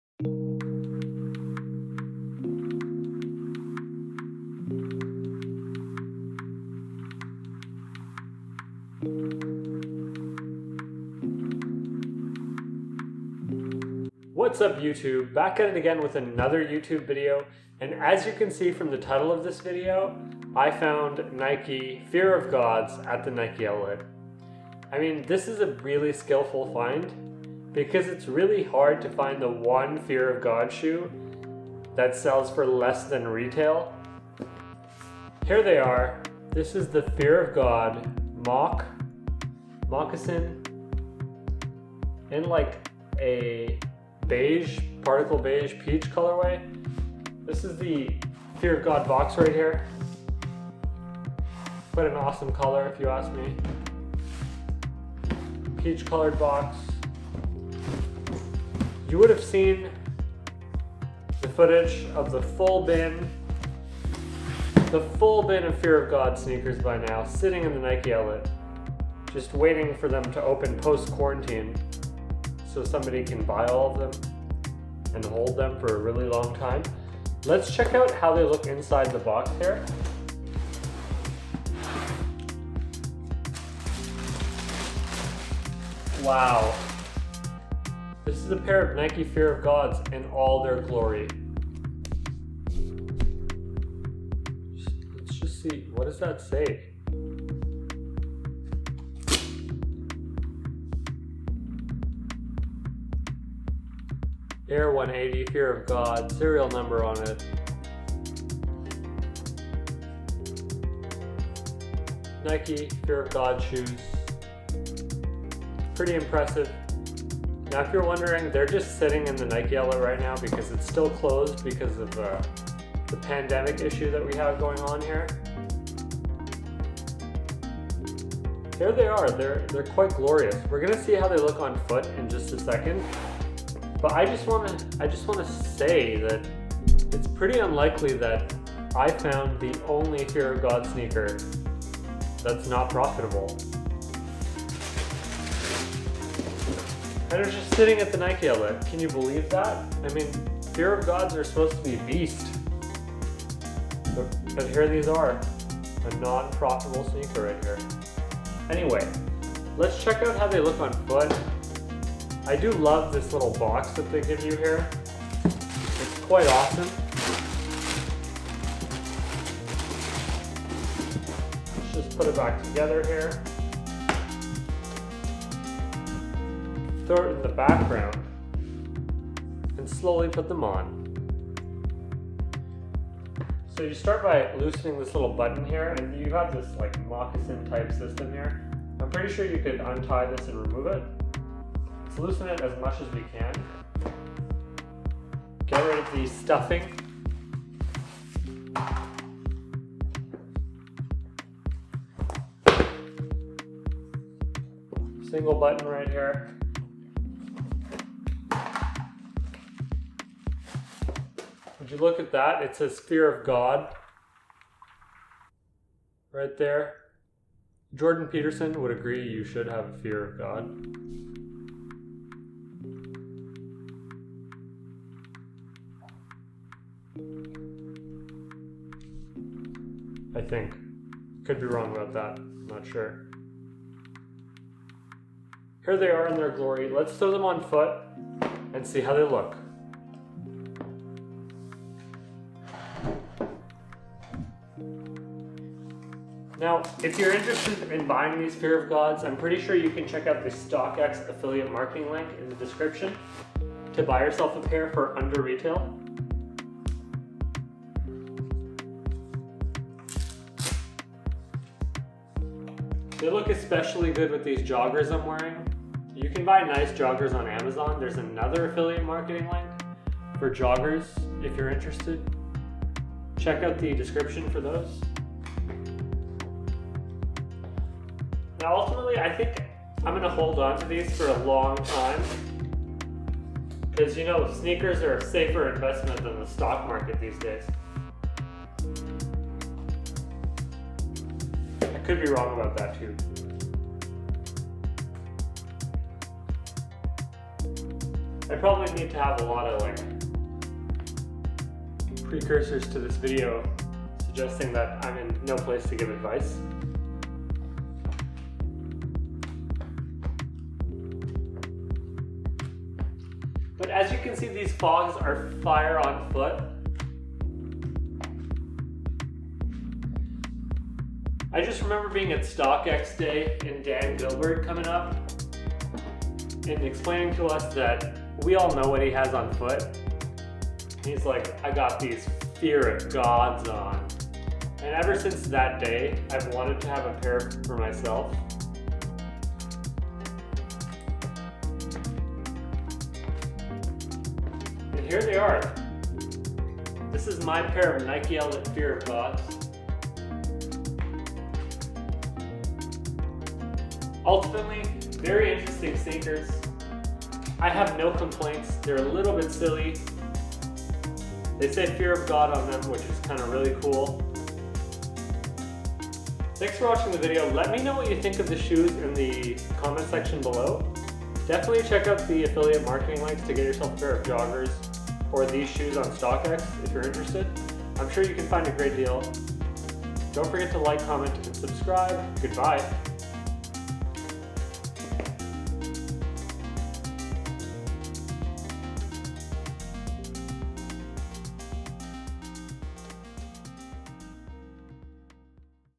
I found Fear Of God's at the Nike Outlet: Nike Air Fear Of God Moccasin Particle Beige
If you are interested in the joggers check it out here:
USA: